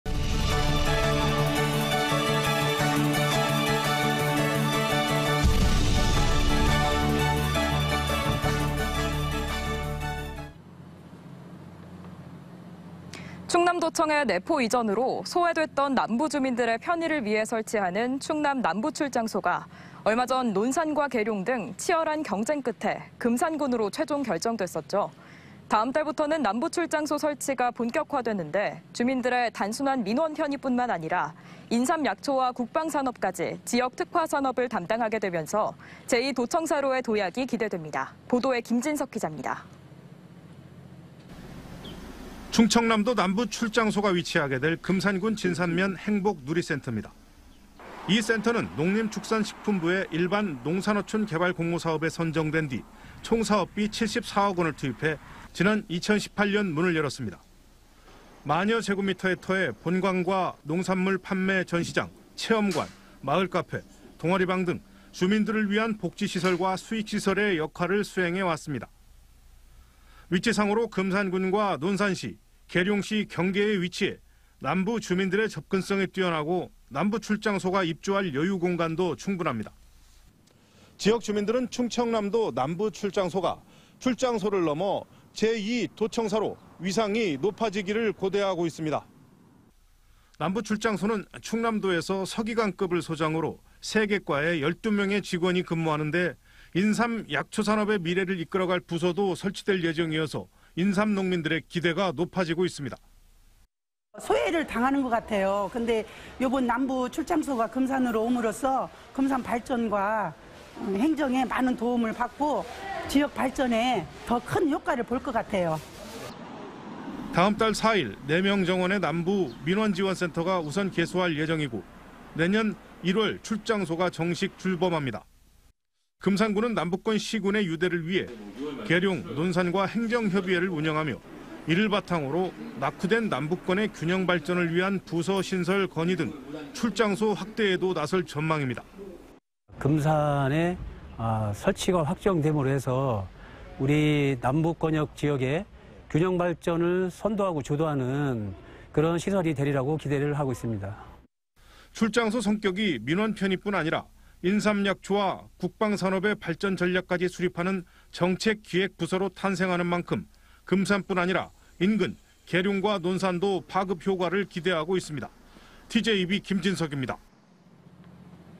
충남 남부출장소, 다음 달 4일 임시 개소.. '제2 도청사 기대 모아'｜ TJB 대전·세종·충남뉴스
【 앵커멘트 】

충남도청의 내포 이전으로
소외됐던 남부 주민들의
편의를 위해 설치하는
충남 남부출장소가
얼마 전 논산과 계룡 등
치열한 경쟁 끝에
금산군으로 최종 결정됐었죠.

다음달부터는 남부출장소
설치가 본격화되는데,
주민들의
단순한 민원 편의 뿐만 아니라
인삼약초와 국방산업까지
지역특화산업을 담당하게 되면서
제2도청사로의 도약이 기대됩니다.

보도에 김진석 기자입니다.